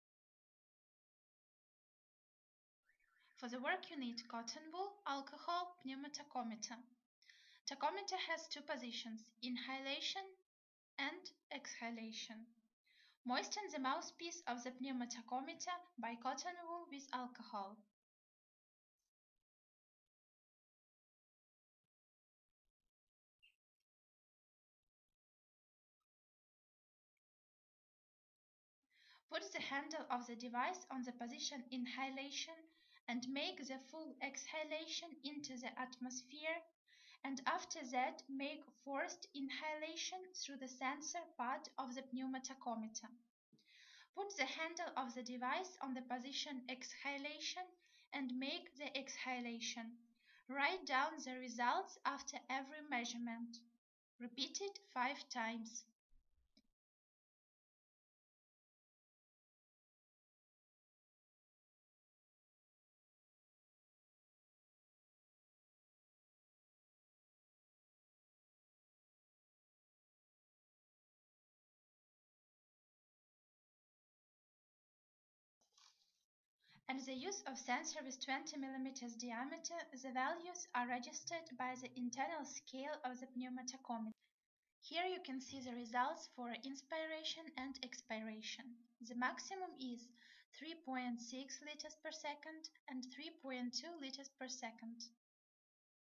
Pneumotachometry [HD]
Pneumotachometry is the method of determination of air peak floe during forced inspiration- the maximum volume rates of inhalation and forced expiration.